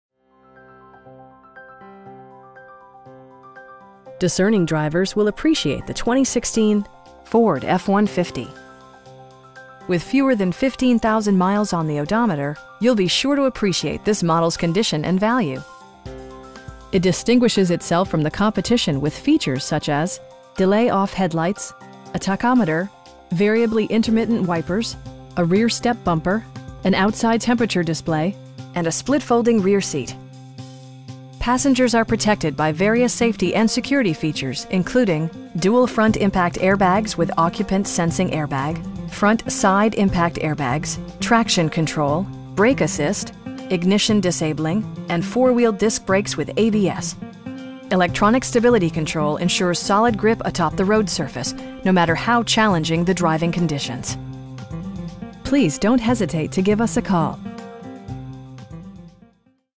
2016 Ford F-150 in Moline, IL 61265
Sexton Ford
3802 16th Street

Discerning drivers will appreciate the 2016 Ford F-150. With fewer than 15,000 miles on the odometer, you'll be sure to appreciate this model's condition and value. It distinguishes itself from the competition with features such as: delay-off headlights, a tachometer, variably intermittent wipers, a rear step bumper, an outside temperature display, tilt and telescoping steering wheel, and a split folding rear seat. Passengers are protected by various safety and security features, including: dual front impact airbags with occupant sensing airbag, front SIDE impact airbags, traction control, brake assist, ignition disabling, and 4 wheel disc brakes with ABS. Electronic stability control ensures solid grip atop the road surface, no matter how challenging the driving conditions. We know that you have high expectations, and we enjoy the challenge of meeting and exceeding them! Please don't hesitate to give us a call.